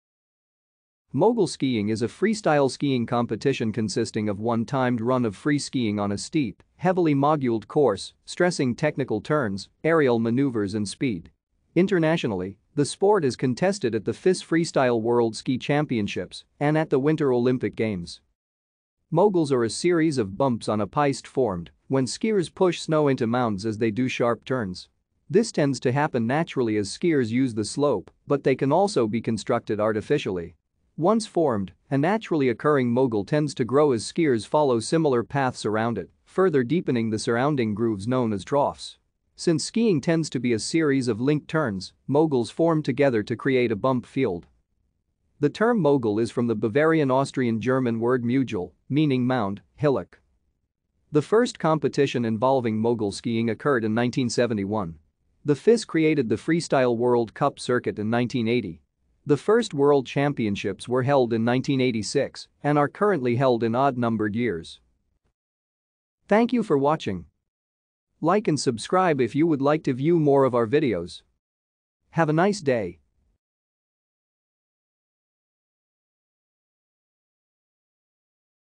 Mogul Skiing
Mogul skiing is a freestyle skiing competition consisting of one timed run of free skiing on a steep, heavily moguled course, stressing technical turns, aerial maneuvers and speed. Internationally, the sport is contested at the FIS Freestyle World Ski Championships, and at the Winter Olympic Games.